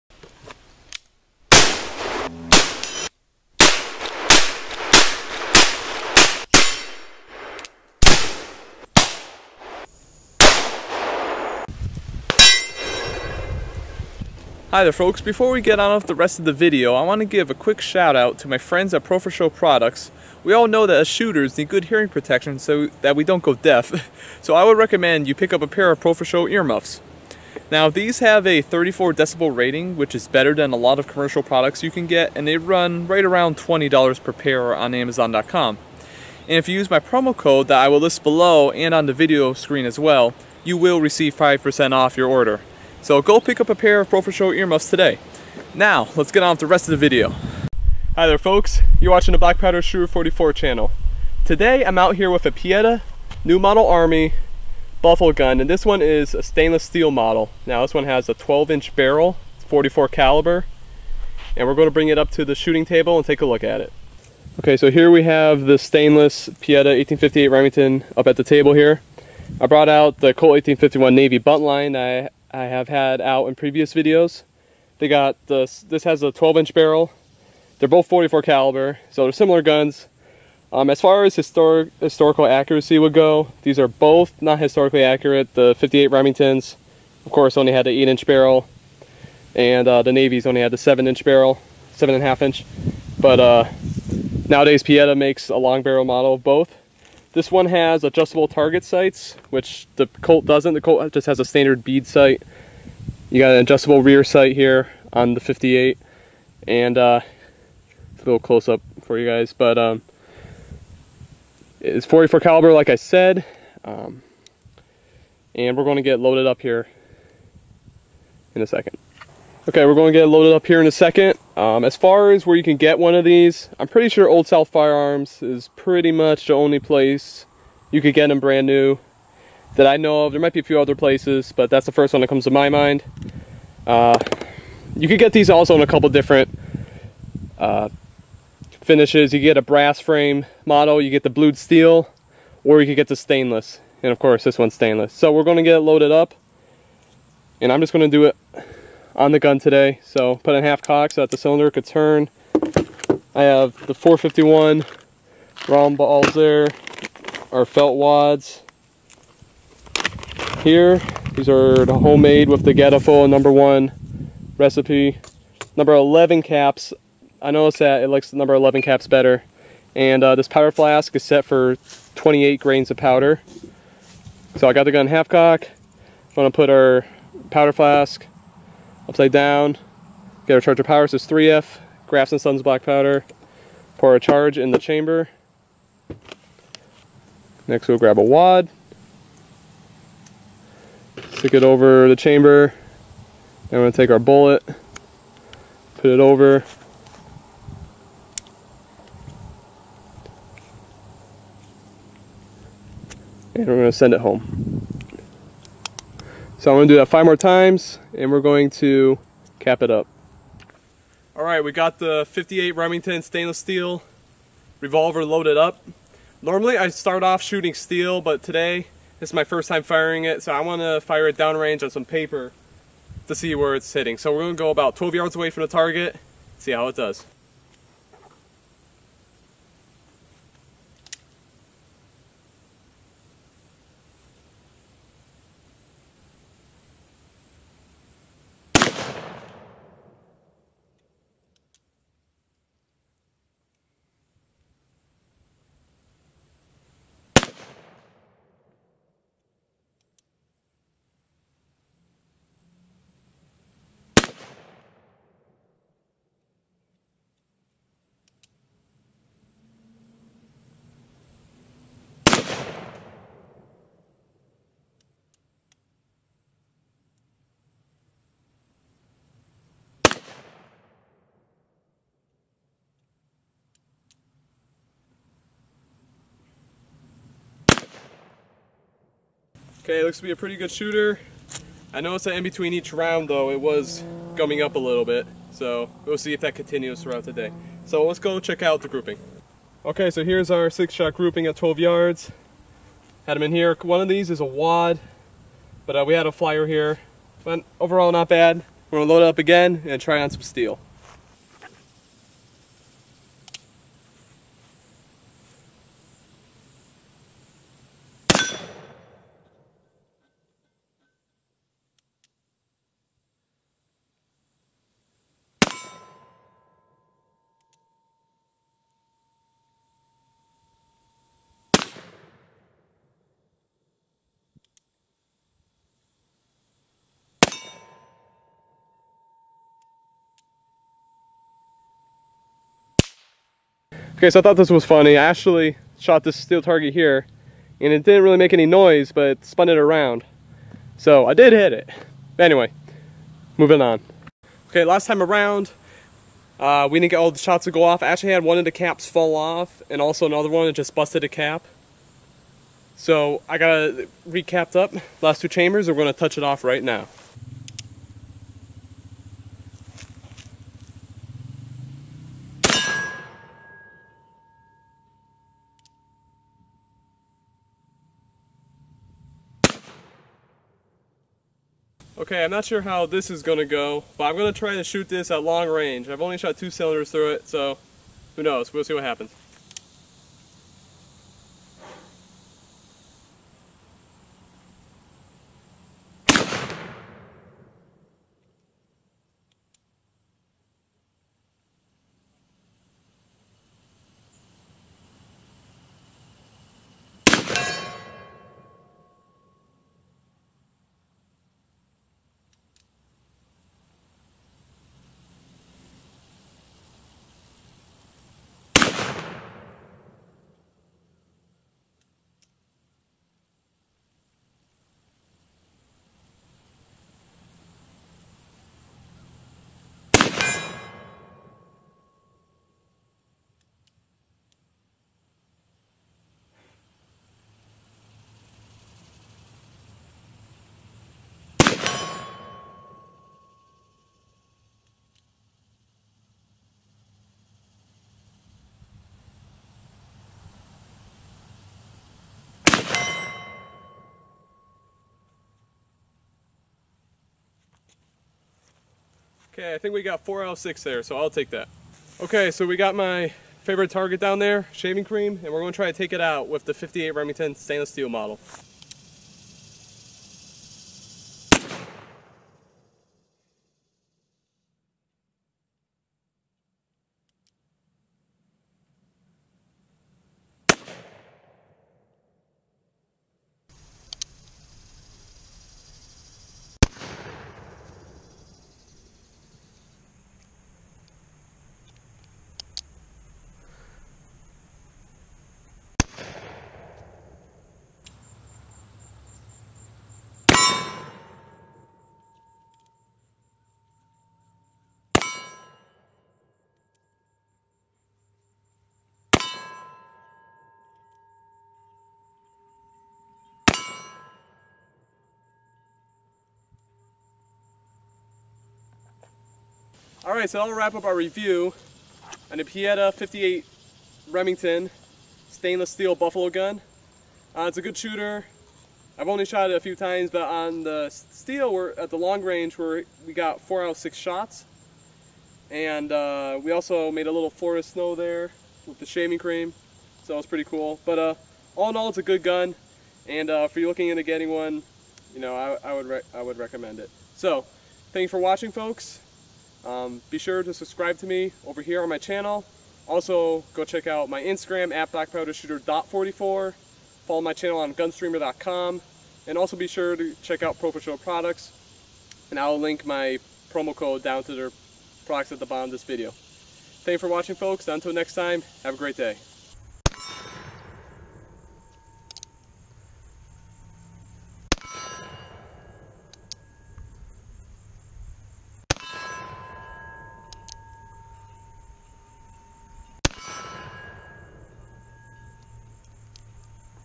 Shooting The Pietta 1858 .44 Stainless Steel 12" Buffalo Black Powder Revolver
In this video we'll bring a Pietta 1858 Stainless Steel Buffalo model cap and ball revolver out to the range.